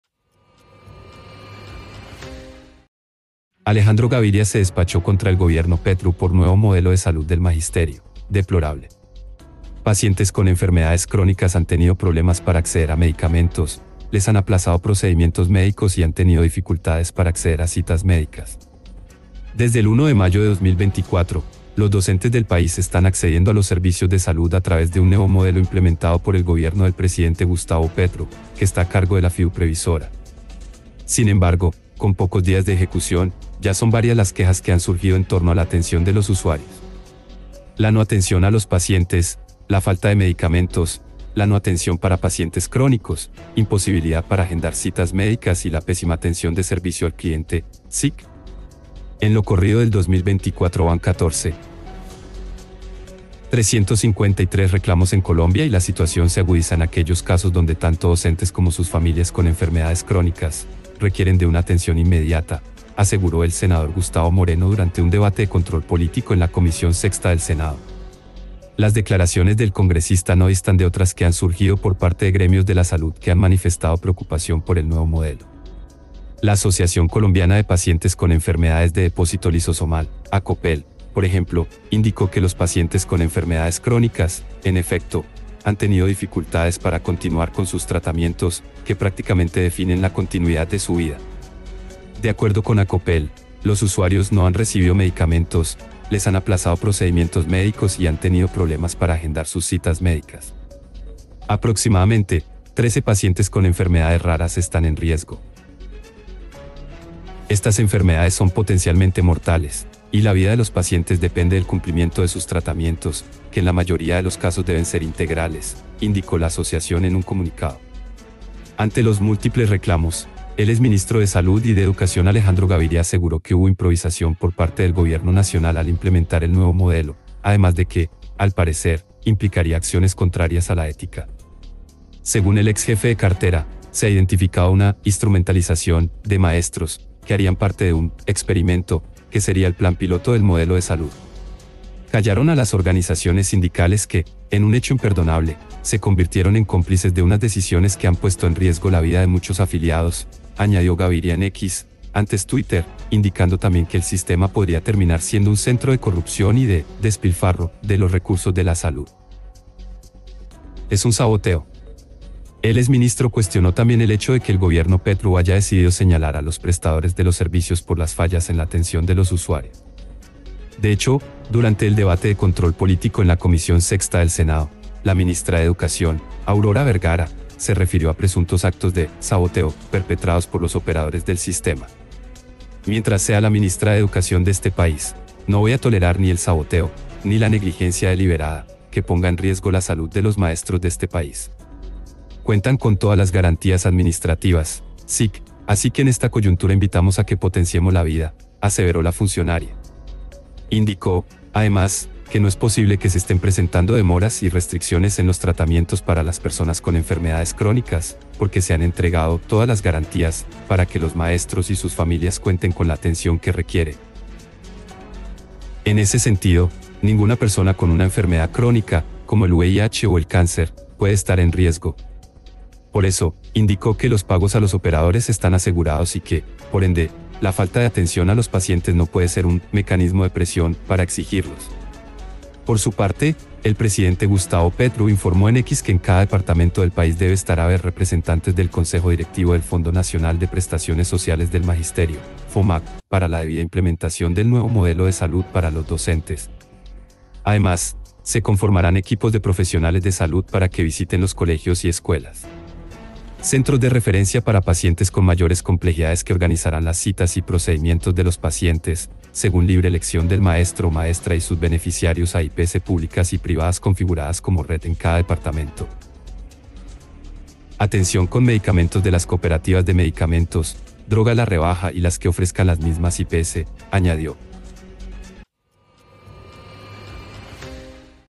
¡DEPRORABLE! ⚫ Alejandro Gaviria se despachó contra el Gobierno Petro por nuevo modelo de salud
Alejandro Gaviria se despachó contra el Gobierno Petro por nuevo modelo de salud del magisterio: “Deplorable”
Pacientes con enfermedades crónicas han tenido problemas para acceder a medicamentos, les han aplazado procedimientos médicos y han tenido dificultades para acceder a citas médicas
INFOBAE